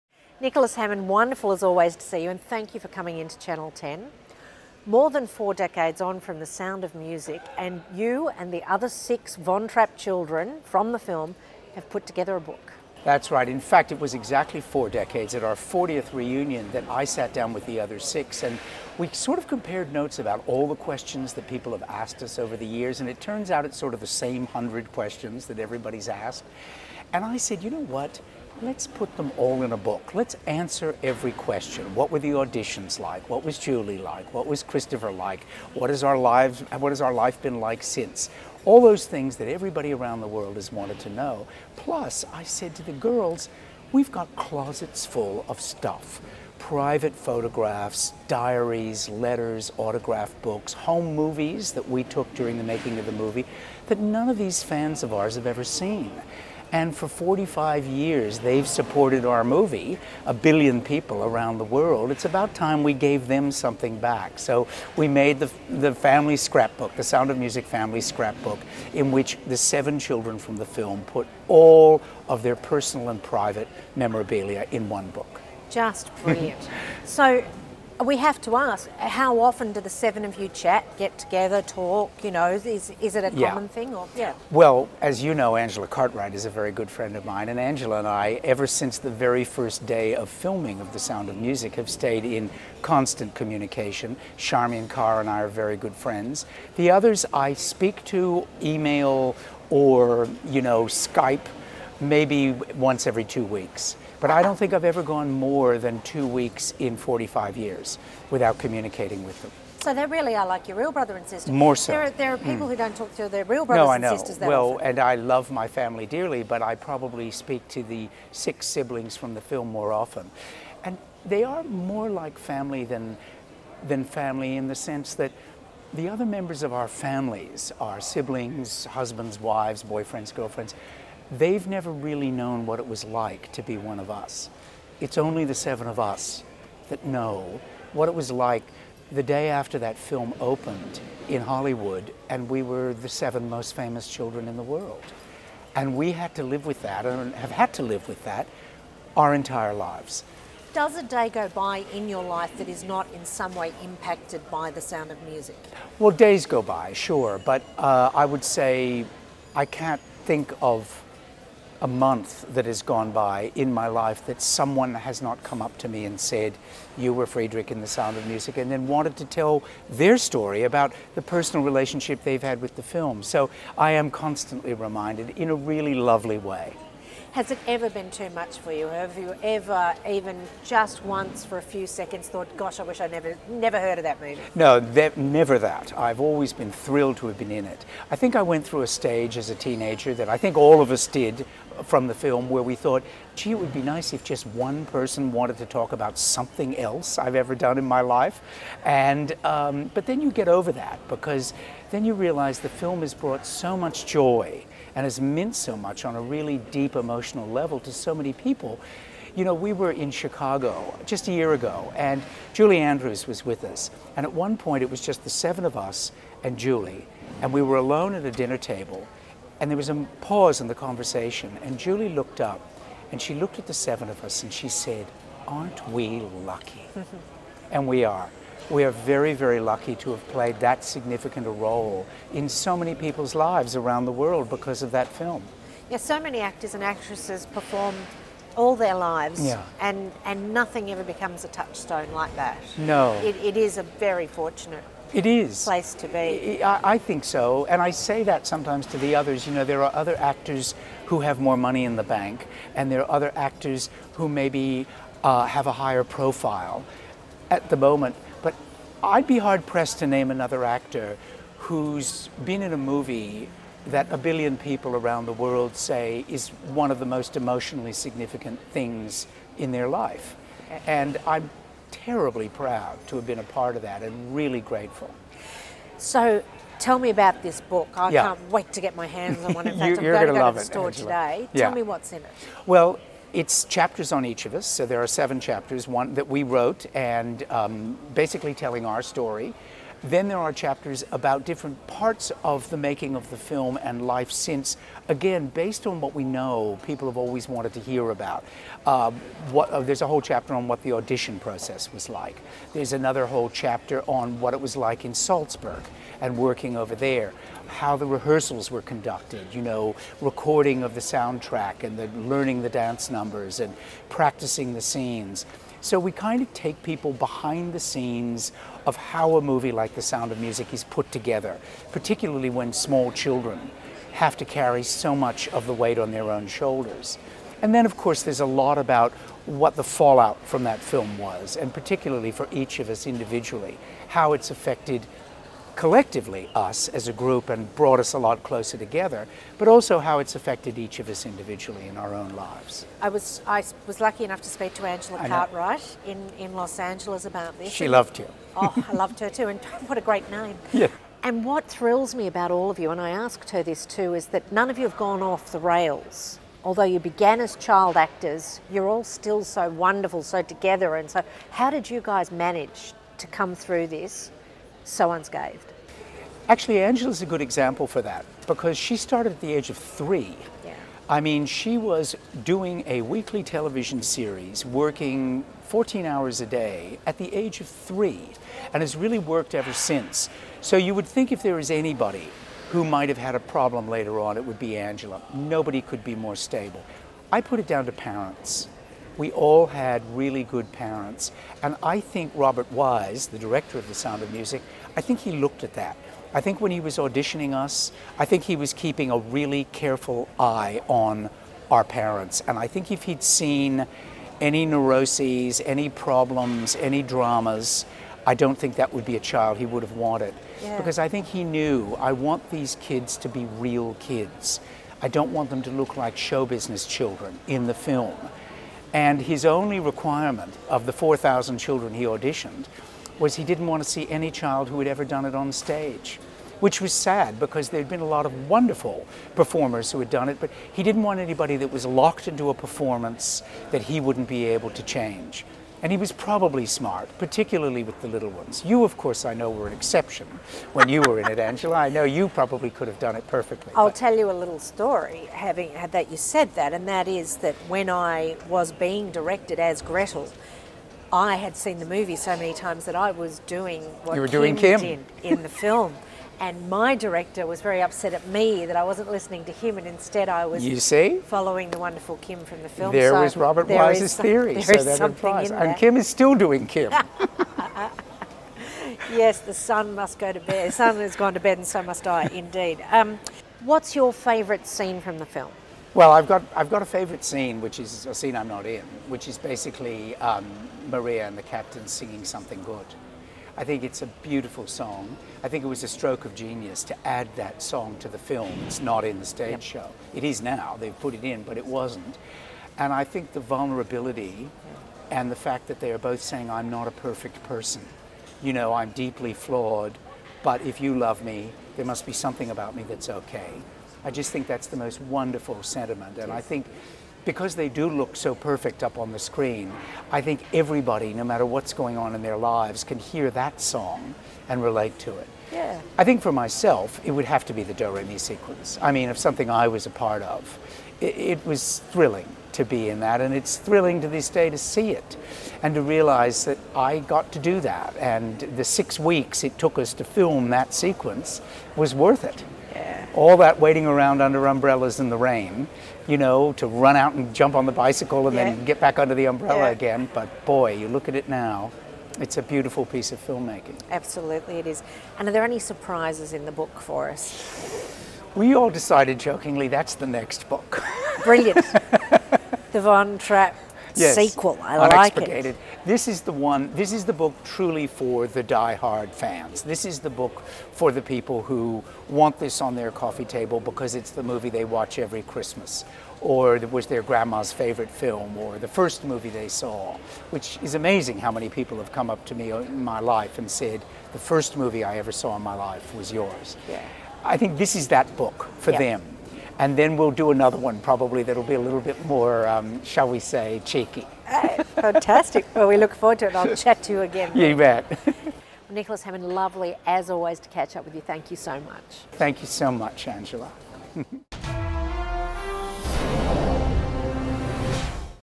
Nicholas Hammond interview
Angela Bishop's complete interview with Sound of Music star Nicholas Hammond.